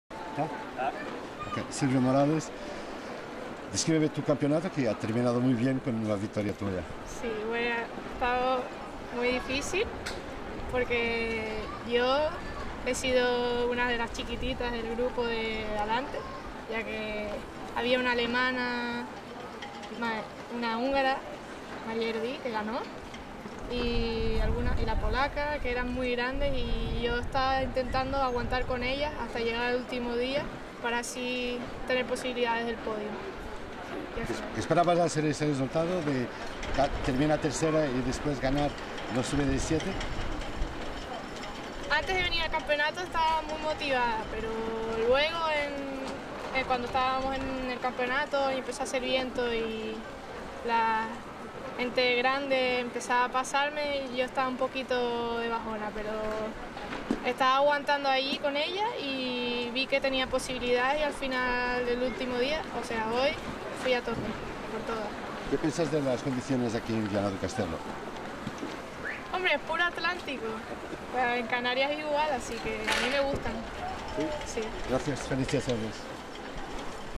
150725 SILVIA MORALES EUROPEO JUVENIL LASER RADIAL VIANA DO CASTELO
La tinerfeña Silvia Morales ha logrado finalizar en tercera posición tras la húngara Ardi y la polaca Magdalena Kwasna, después de doce pruebas durante todo el Europeo Juvenil OPEN de Laser Radial; alcanzando el mejor resultado entre toda la representación española. Morales ha luchado mucho por este bronce en aguas de Viana do Castelo, donde las condiciones meteorológicas y de mar han sido muy variadas, la mayor de las veces con fuertes vientos atlánticos de componente noroeste.